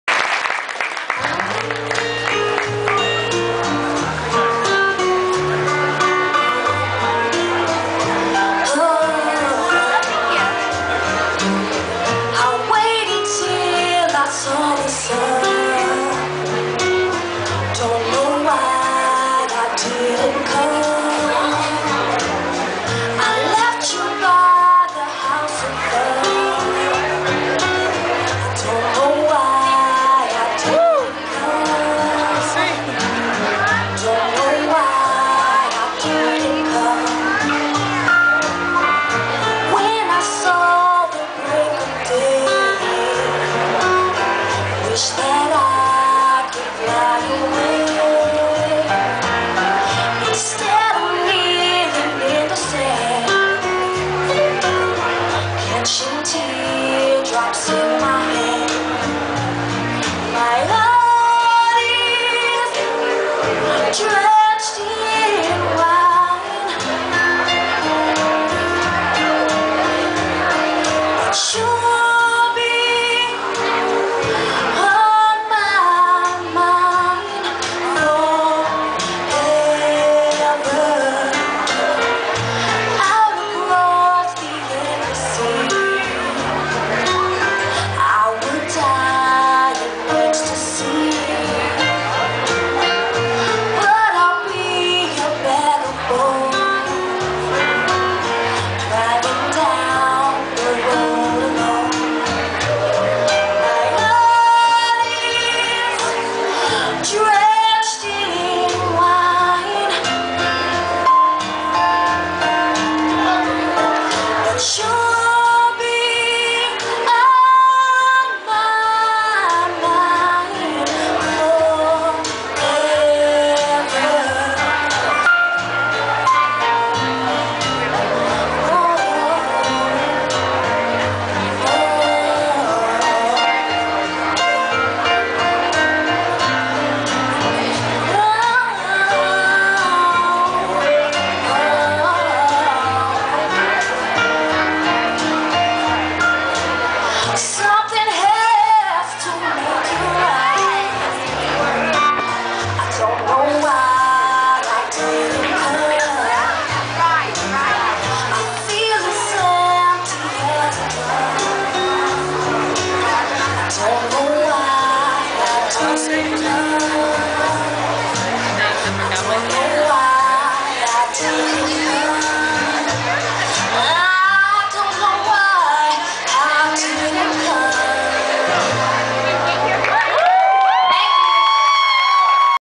Laivan Greene performs Kids With A Cause benefit concert
Laivan Greene gives a performance at the knitting factory in hollywood california.